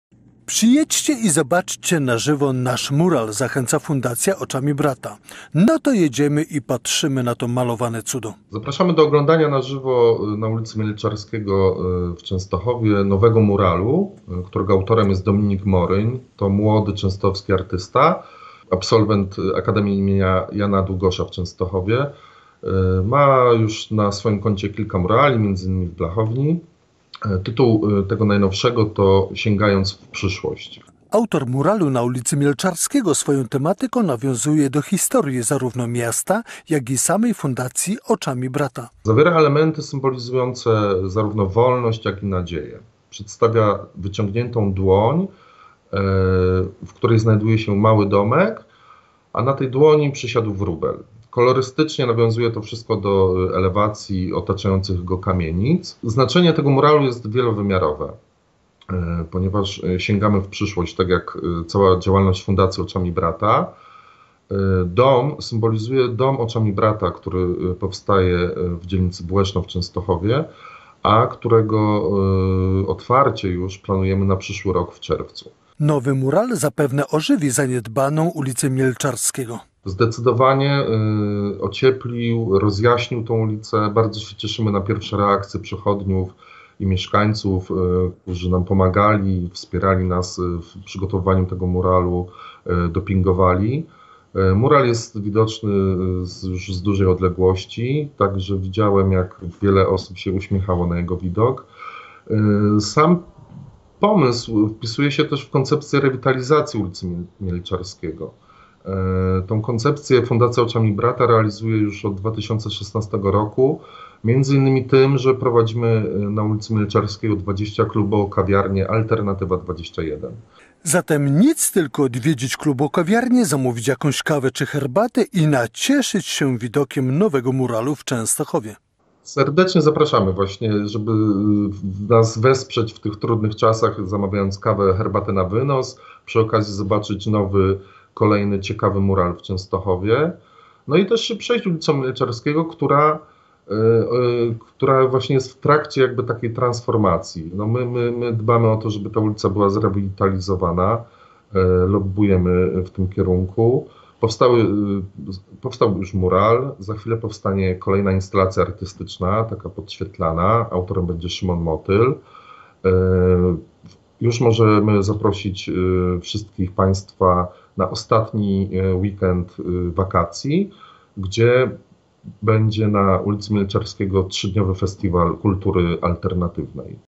Muralem sięgnęli w przyszłość
Mamy to! Nowy mural pod nazwą "Sięgając w przyszłość" jest już gotowy - chwali się Fundacja Oczami Brata.